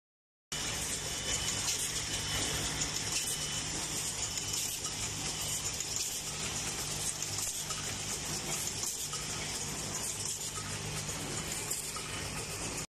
Production line of irrigation dripper
With CE certified adjustable irrigation dripper, it has a mature mechanized production line .